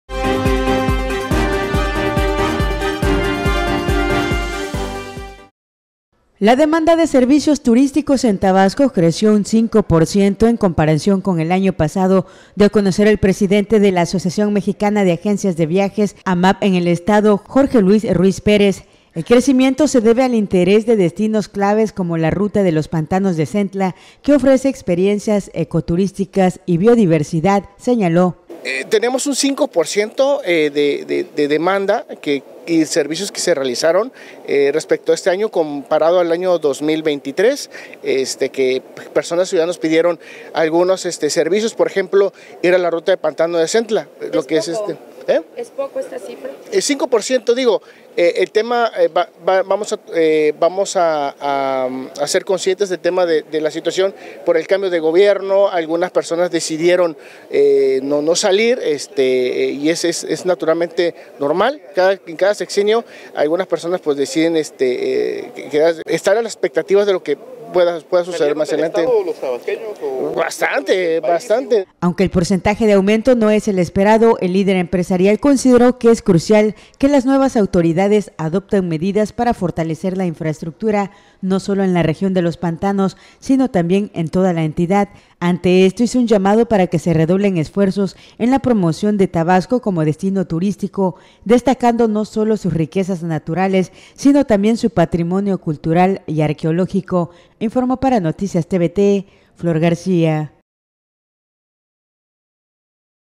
Registra Tabasco un aumento del 5% en demanda de servicios turísticos
Creció cinco por ciento la demanda de servicios turísticos en Tabasco en comparación con el año pasado, resalta el presidente de la Asociación Mexicana de Agencias de Viajes en el Estado, Jorge Luis Ruiz.